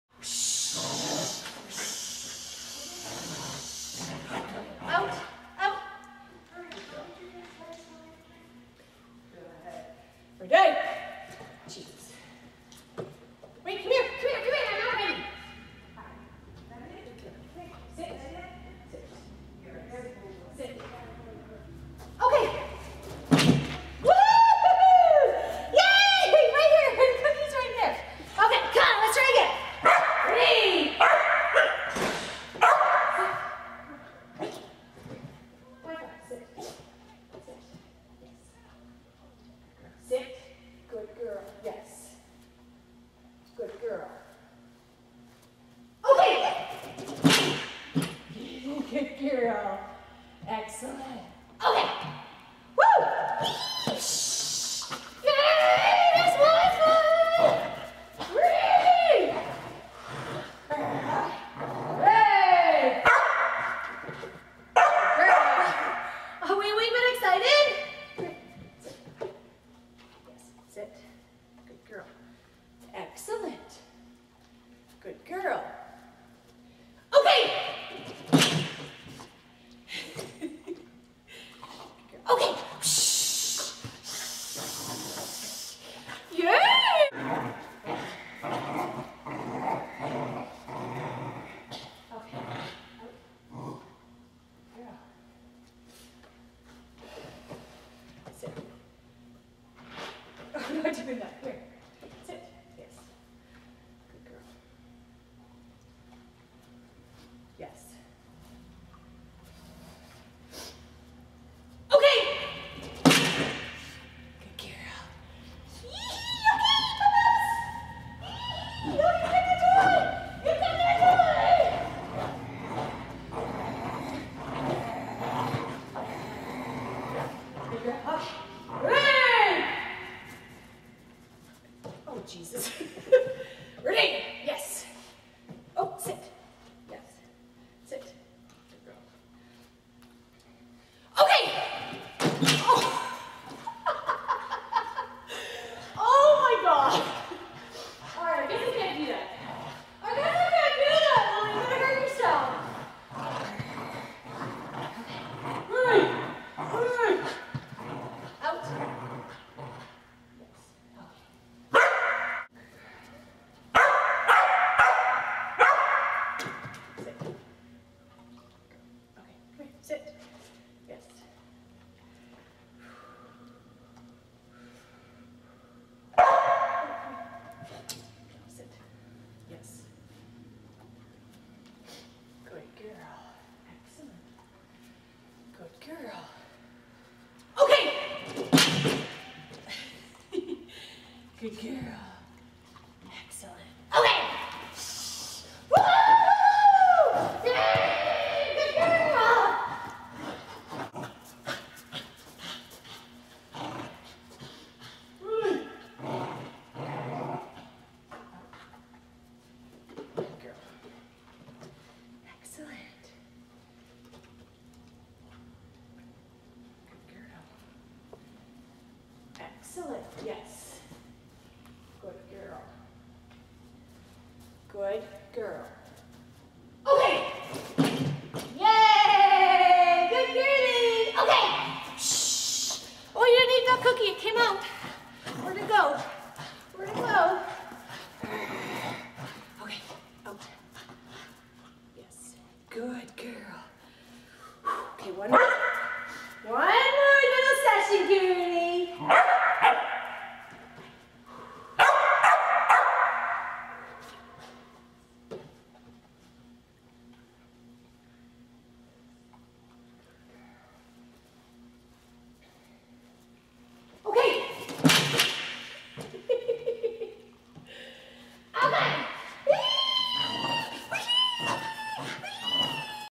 WiFi Training-Feb 2012
WiFi on some early See-saw training. She is a blast to train: bold and very sassy!